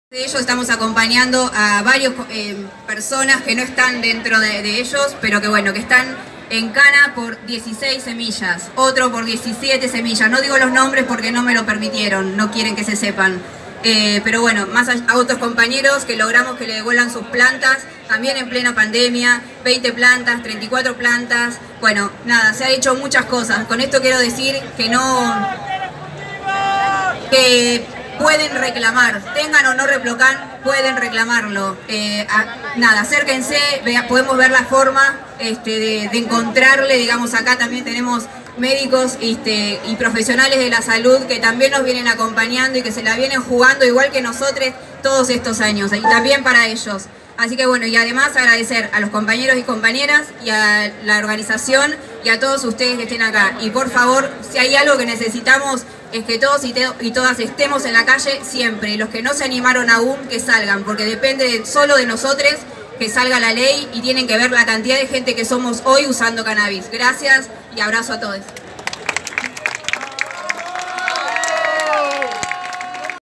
24 de noviembre, hoy al Congreso liberén al cannabis
Seguimos reclamando , pidiendo , rogando  libertad , justicia , igualdad desde el año 1999 comenzarón estas marchas de protestas ya van por casi 23 años , cuando me dí cuenta q nada cambiaba  en tanto tiempo  decidí hace un par de años q  los iba ayudar  con los dones q Dios me ha otorgado y se liberó en muchos lugares pero no puedo abrir esta gran  puerta yo sola por eso si sos del palo vení al congreso, cada uno de nosotros podemos ser los próximos en caer en la torre de la bastilla .
La Torre de la Bastilla era donde estaban los presos en la época de la revilución francesa el pueblo muerto de hambre se reveló y no se porque extraña sensación me parece q seguimos en esa época seguramente es por la misma sensación de opresión recuerden q las brujas somos muy sensibles . El pueblo unido Jamás será vensido .